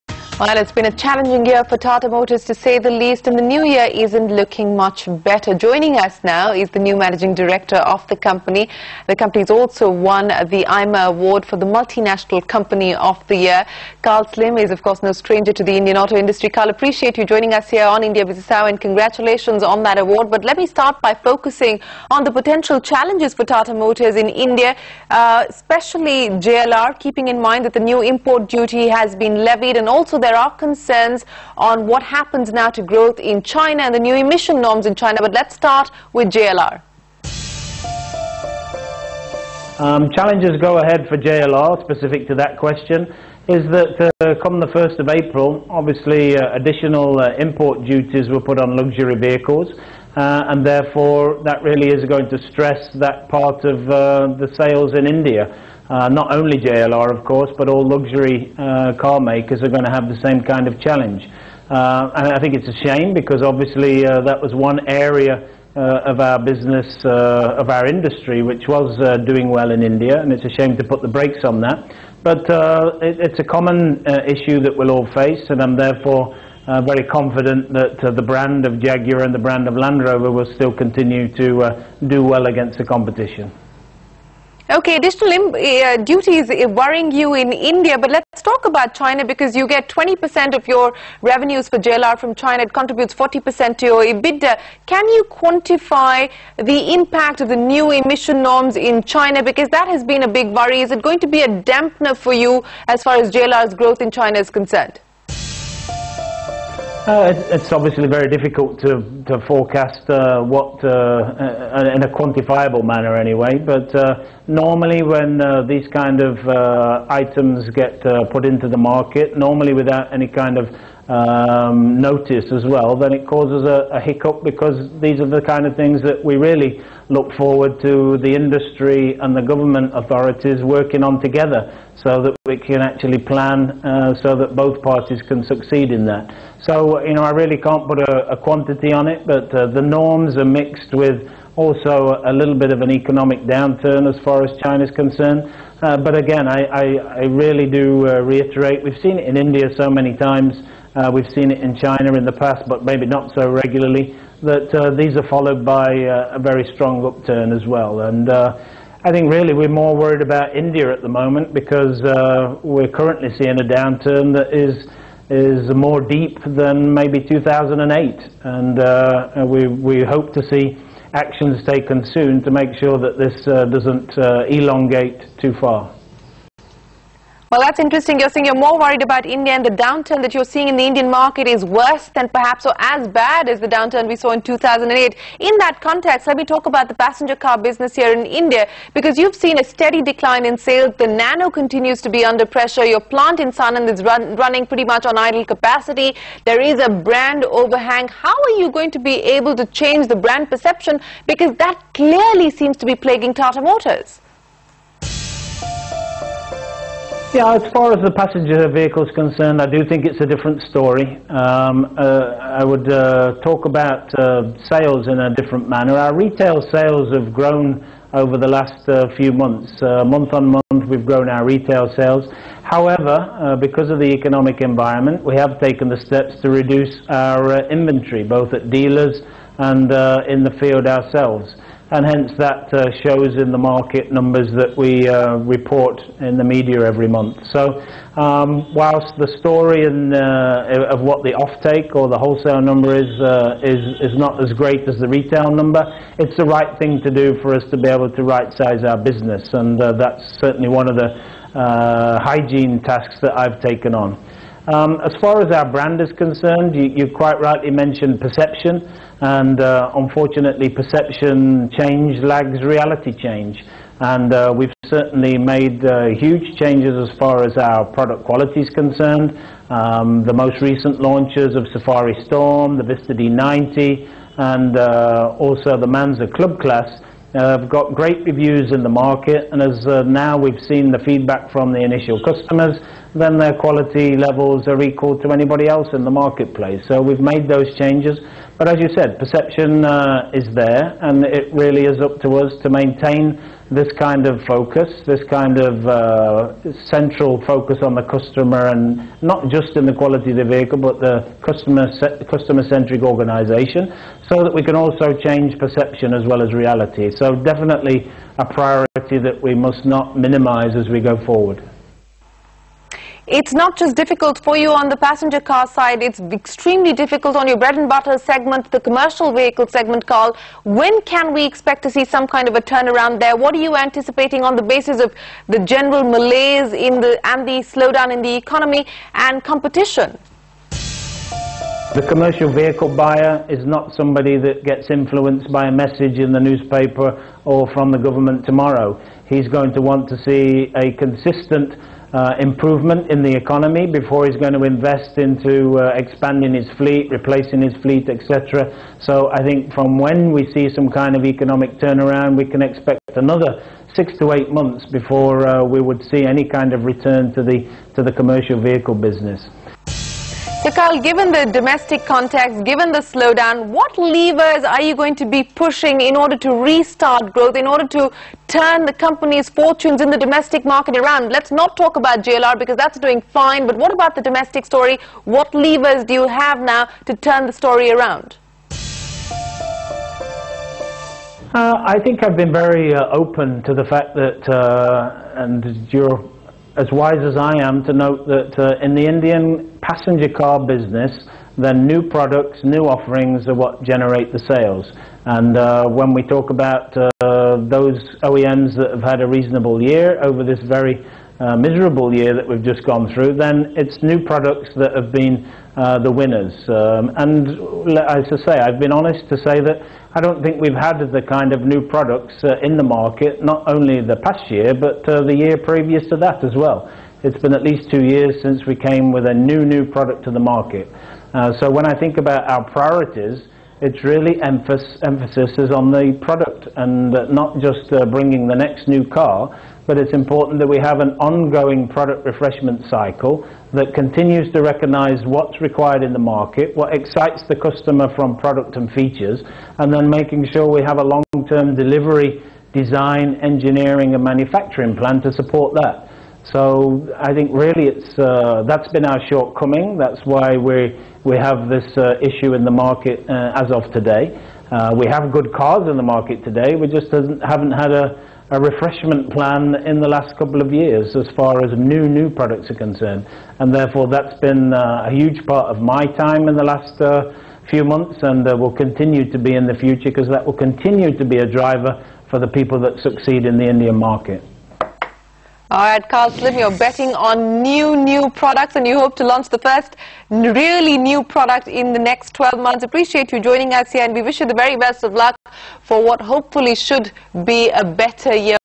India gloomier than China; Tata Motors will be reborn: Slym
Tata Motors MD Karl Slym says, in an interview to CNBC-TV18, that the Indian business environment is gloomier than China and adds that Tata Motors will be reborn with its first product launch in 12 months.